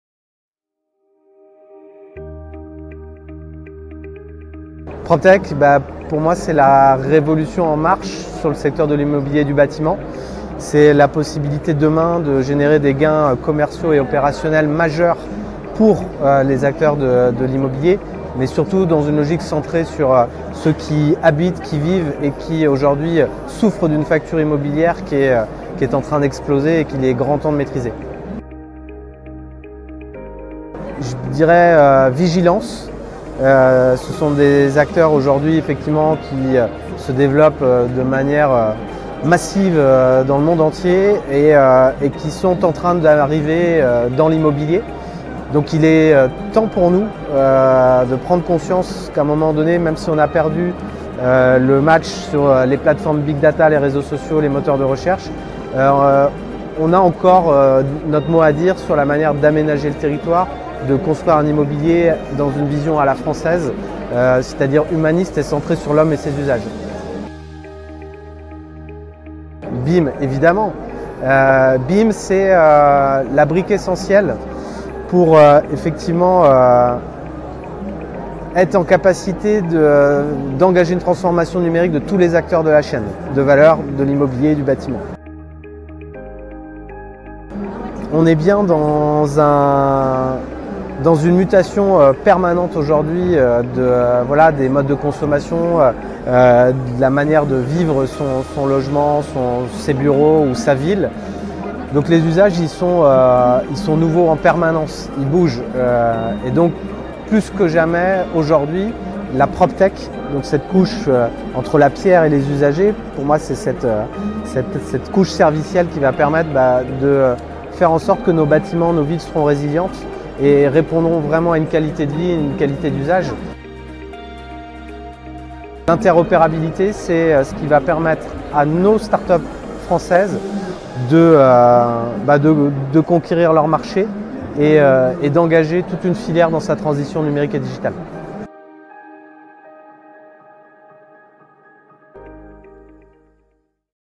L'interview "si je vous dis" : Pierre Leroy, co-fondateur et CEO, EP
Habiteo était présent au salon BIM World qui s'est tenu les 1 et 2 avril dernier à Paris. Une édition riche en temps forts. Au milieu de cet agenda bien chargé, nous avons pris le temps de vous donner la parole ! Alors, Pierre Leroy, co-fondateur et CEO de EP, si je vous dis...